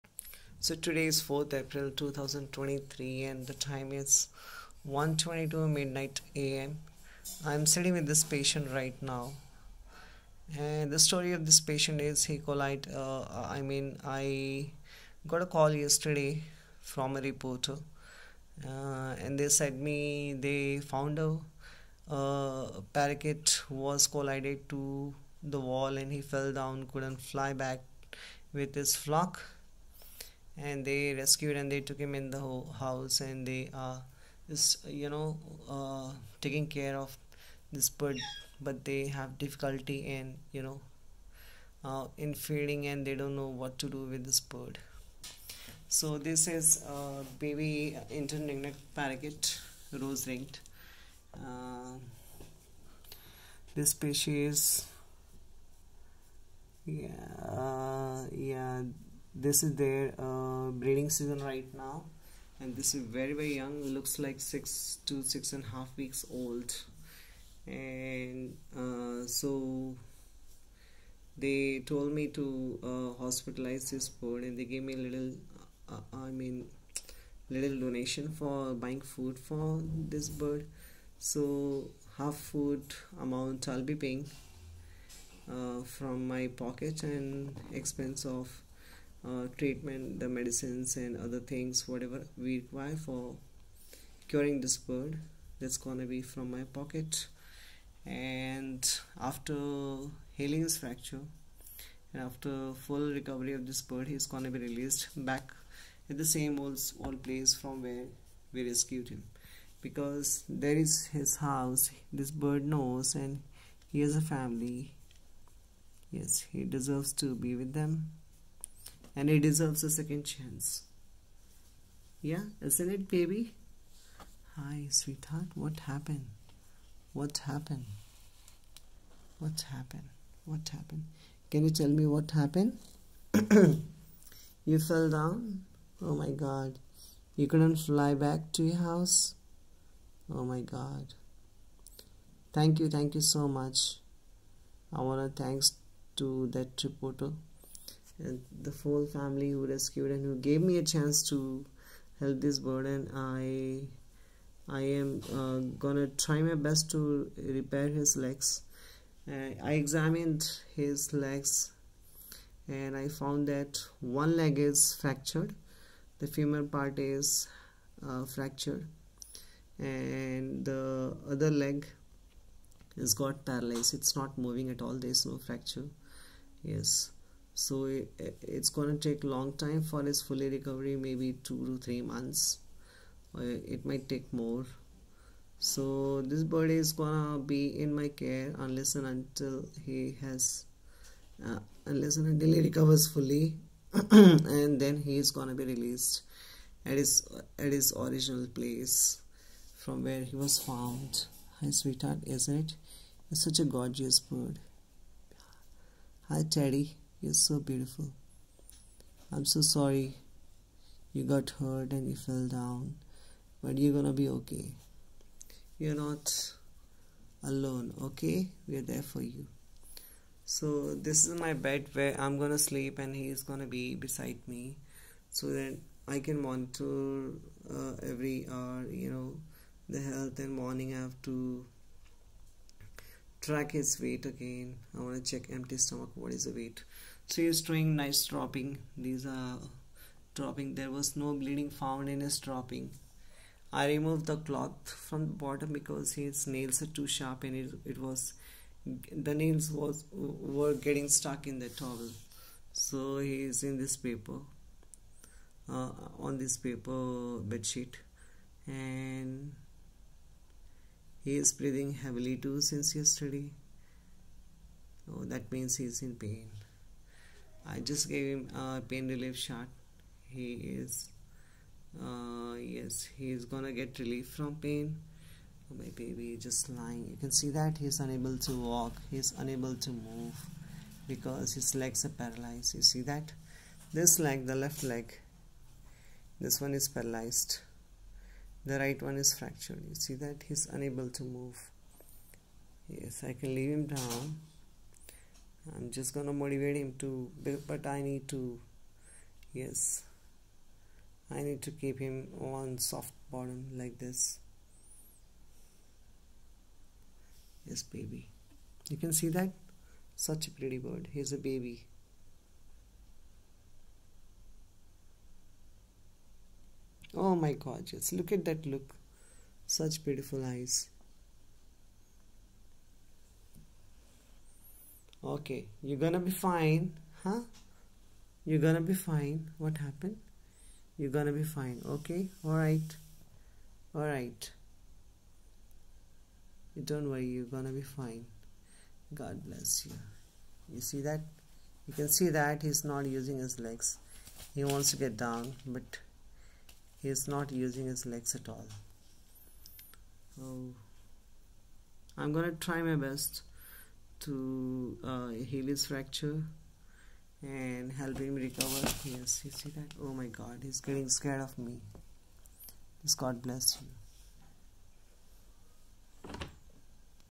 fledgling parakeet collided building and got his leg broken|| bird rescued & receiving medical care
Hi Everyone, I’m a Exotic Bird Specialist who has recently come into the care & treatment of fledgling parakeet that fell 20m from the height after colliding a building wall & was in danger as there were cogs & cats around. He was at risk of getting killed by Predators.
Watching with no sign of parents so we advised the finder/reporter to resq and keep inside home untill he is collected by a rescuer.

I had rehabilitated this little one who is fledgling ringneck parakeet.I got a call from them to resq this bird. He is in my care now, he will receive full medical and supportive care by me untill his full recovery.
Rest assured he will receive the correct diet starting.
He is not foraging or eating independently cz his 1leg is fractured and another leg is paralysed temporarily🩷🐦🪶 He can't eat on his own cz he is still a baby bird .
for nest 2-3mnths he just needs to be hand reared and learn how to fly.
I also have been hand feeding an insect mix loaded with vitamins and protein and he is responding well, toileting as normal, sleeping a lot and keeping his temperature warm. Past 7days he has been doing great and I’m hopeful he is going to make it he just needs some time to wean & recover. I would love to release him if possible but I believe we may have bonded and want to put the word out in the case he cannot be wild or fly or independent. 🐦🪶